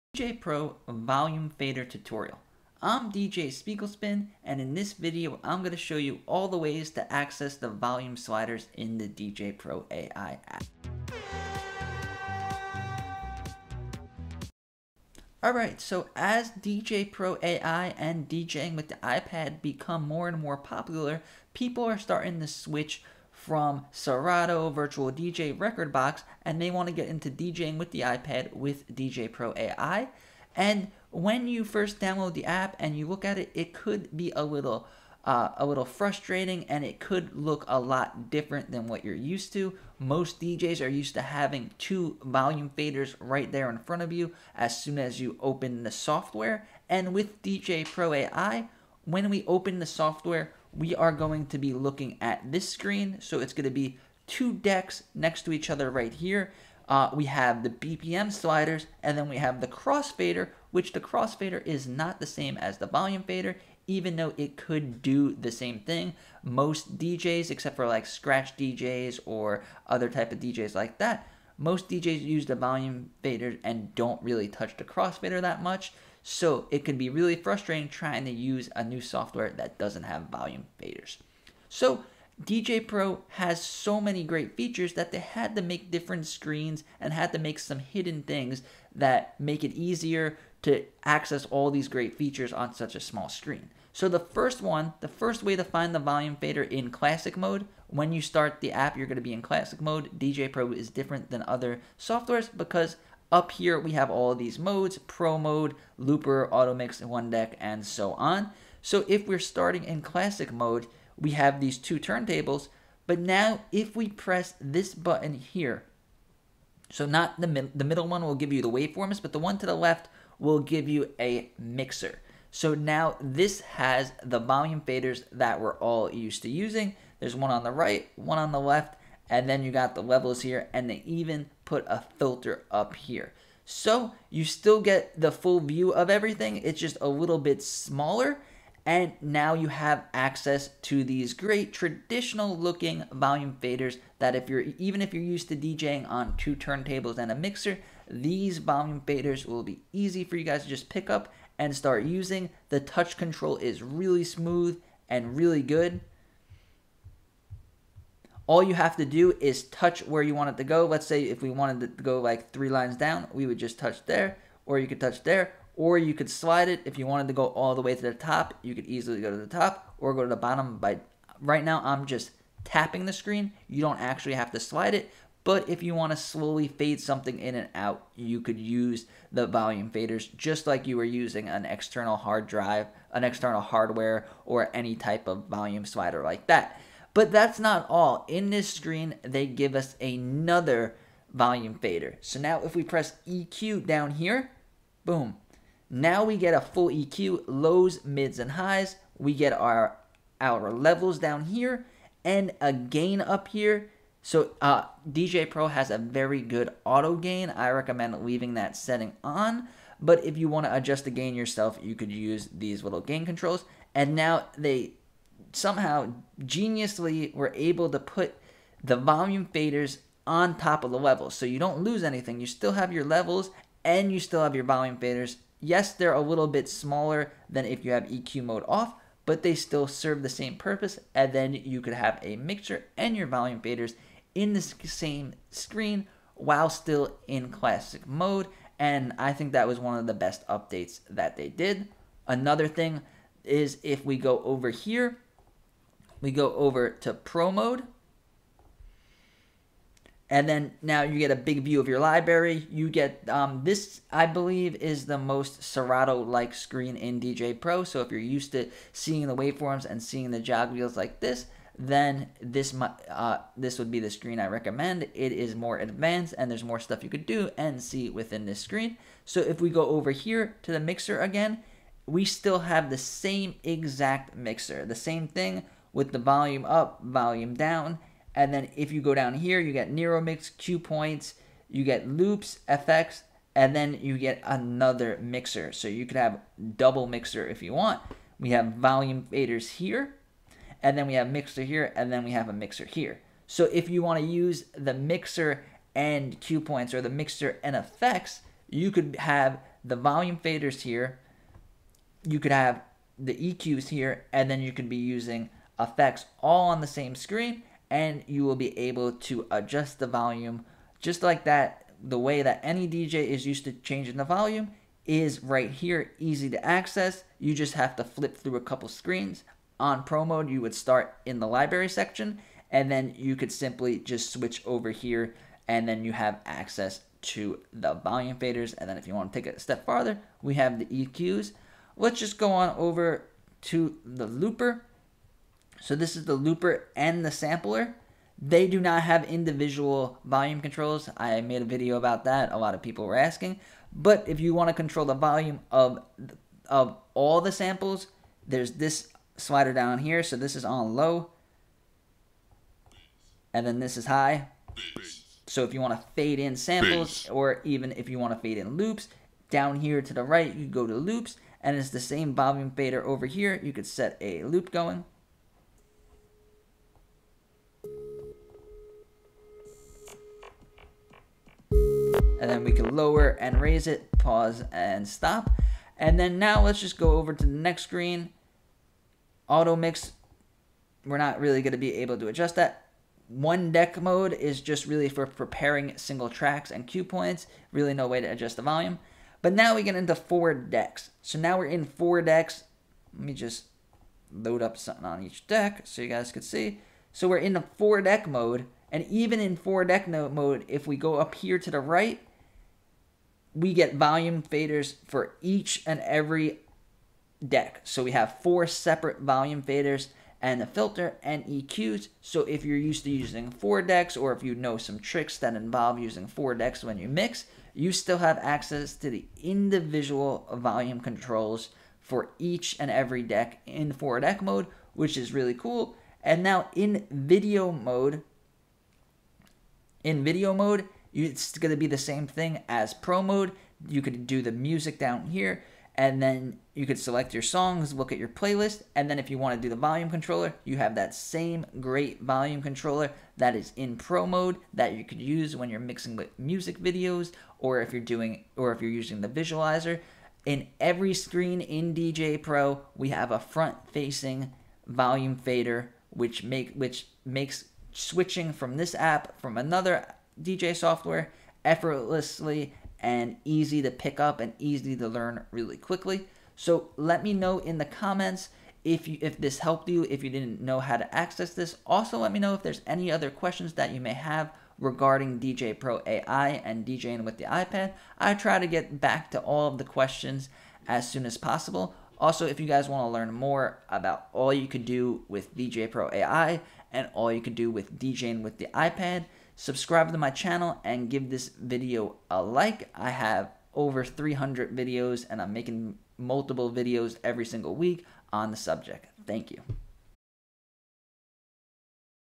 Djay Pro Volume Fader Tutorial
Im DJ Spiegel pin and in this Djay Pro AI tutrorial I show you all the ways to accesses all of the volume faders within the Djay Pro AI app

► Launchpad X Grid Controller

► USB-C Digital AV Multiport Adapter